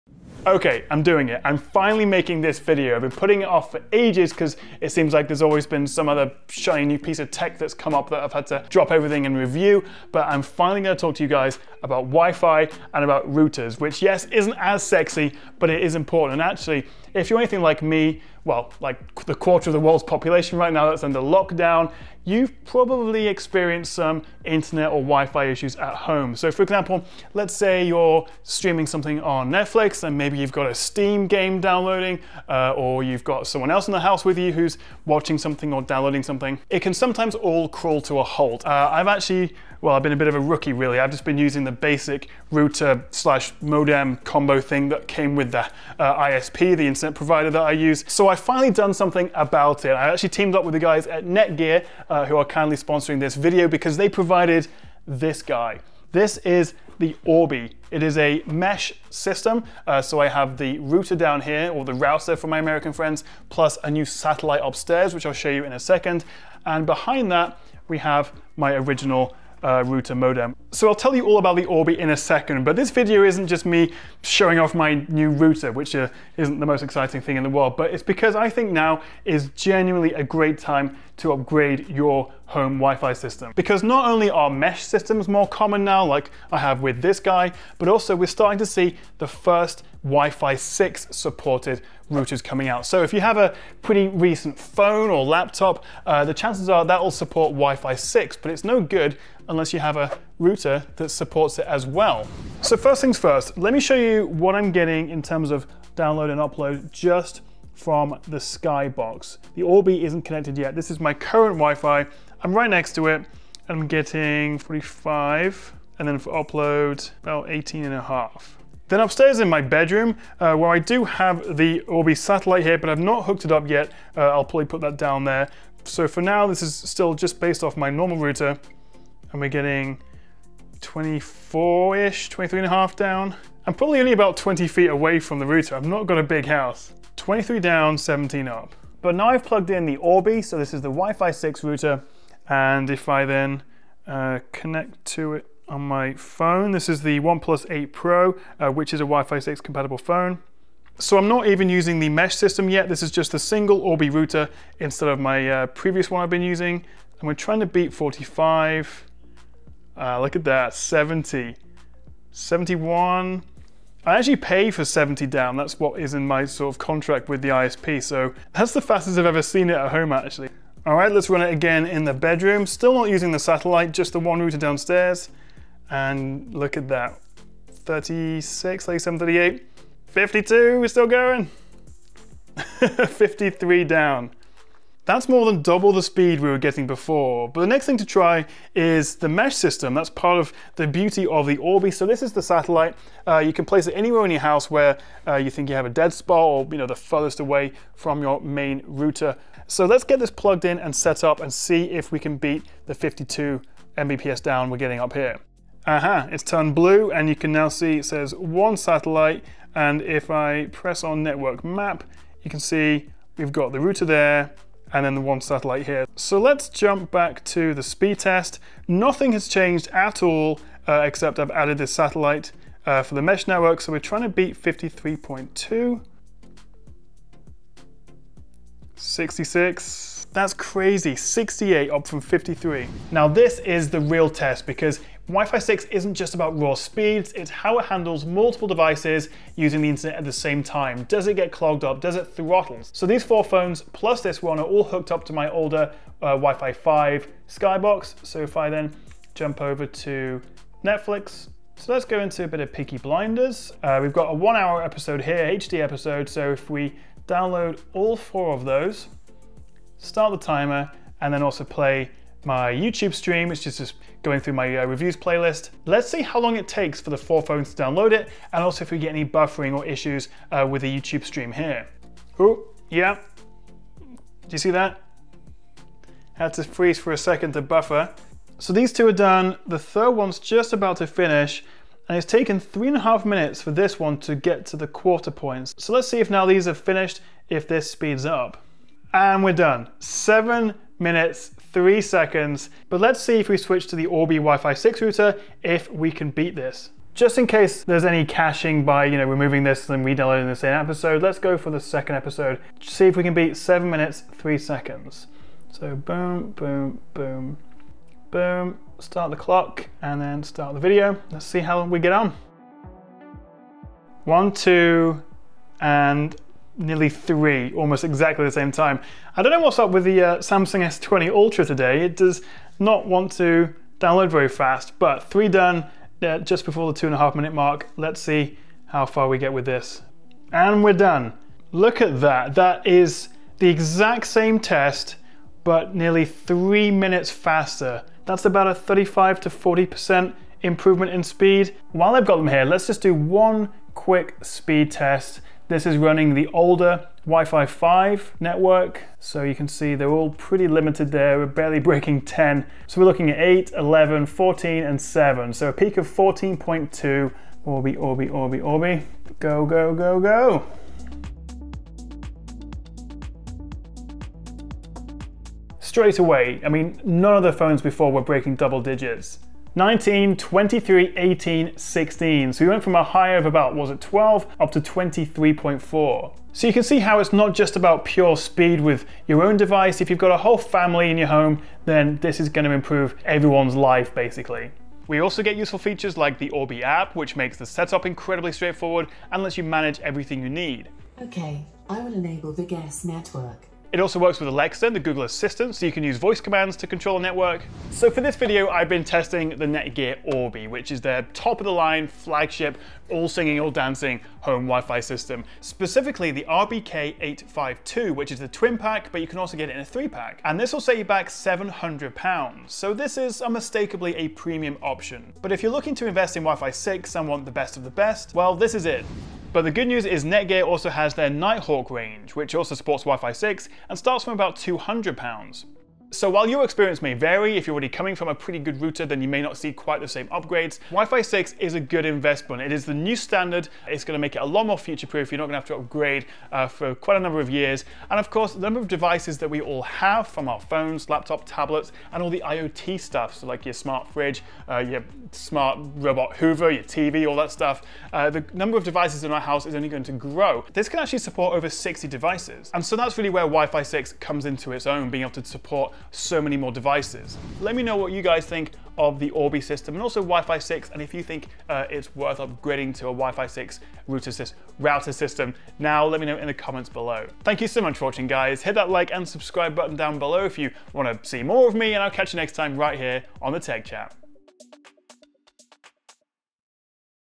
Upgrading to WiFi 6 - What's the Difference? (802.11AX TESTED) | The Tech Chap
AD - With WiFi 6 and Mesh Networks now available - now is a great time to upgrade your home WiFi! But does upgrading your router really make a difference and is it worth it?